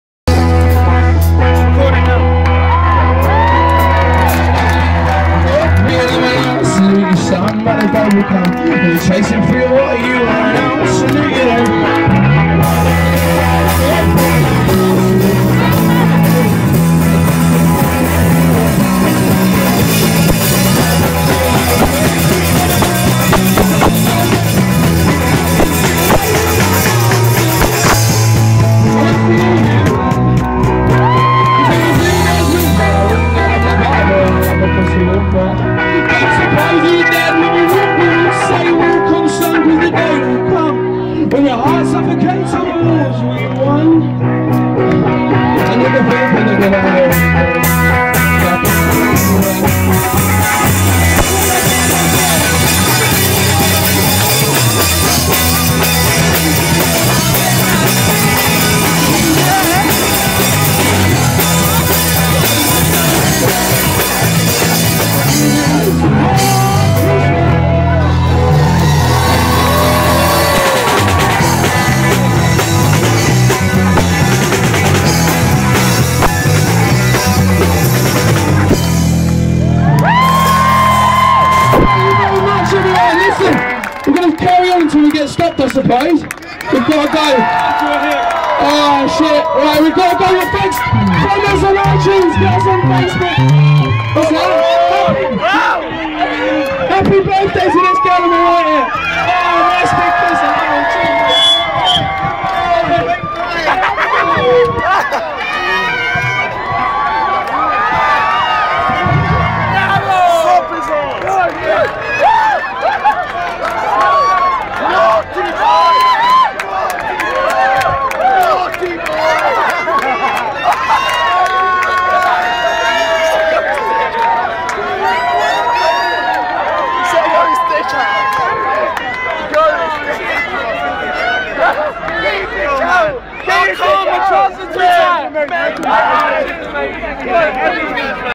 THE RUSKINS - Camden Crawl 2010 STREET RAVE!!
The Ruskins set up on Camden high street and play to over 1000 Camden Crawlers!

TEXT 'TRACK RUSKINS' to 80818 (£1)